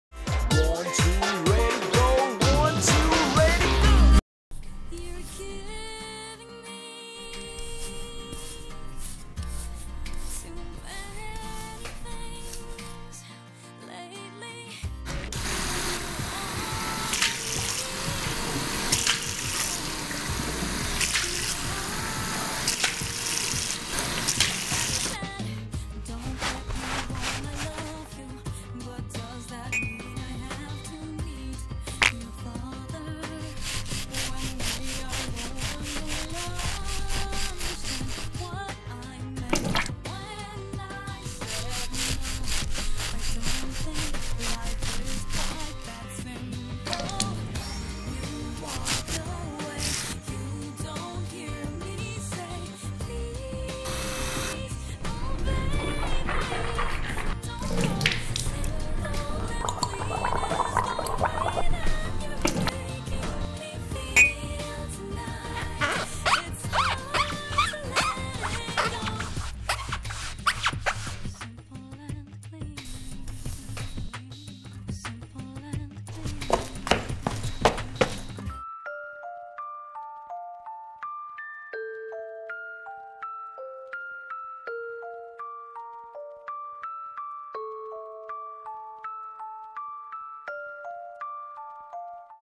The Bathroom
Video for audioclass in school. All sounds are made up and added afterwards.